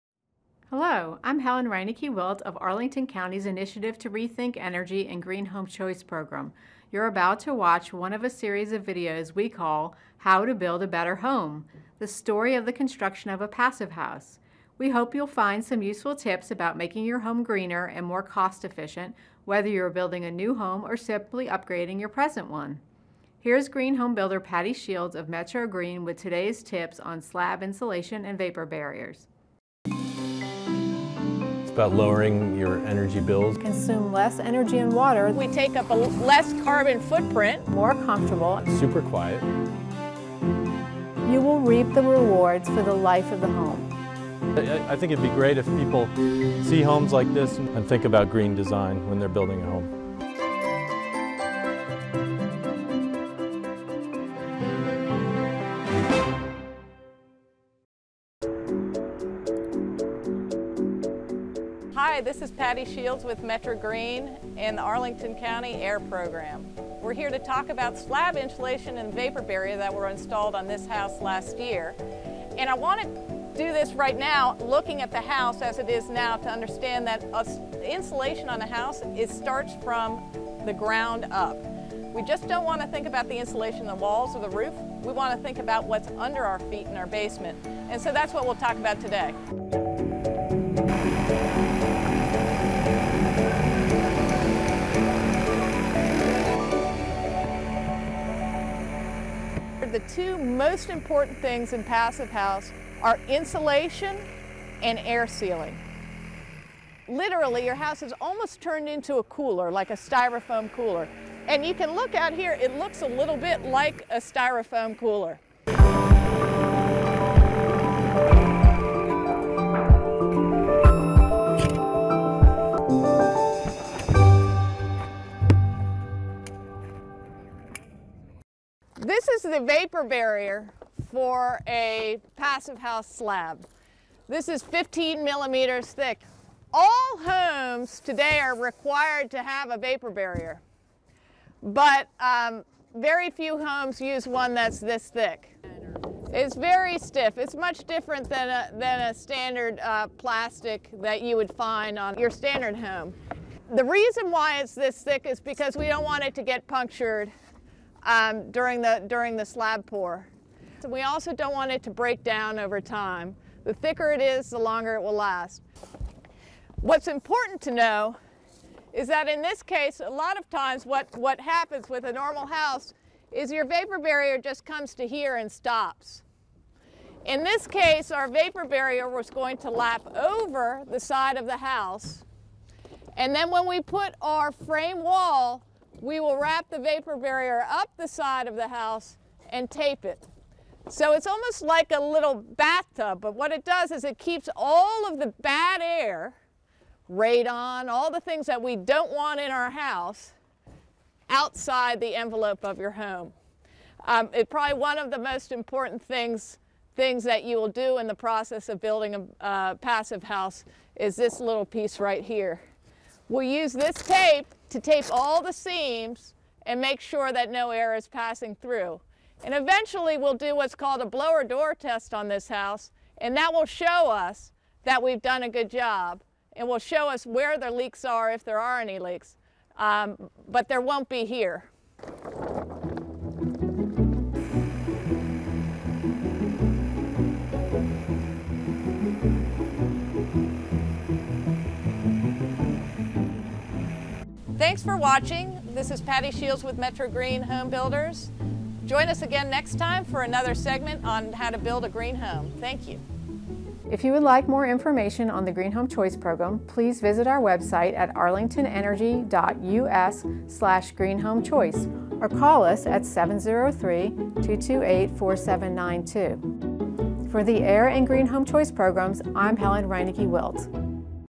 How to Build a Better Home - Slab Insulation and Vapor Barrier
In this segment, green homebuilder Patty Shields talks about the importance of using slab insulation to create a vapor barrier when building a passive house. Arlington County AIRE Green Home Choice program manager Helen Reinecke-Wilt introduces the segment. Links to all nine segments can be found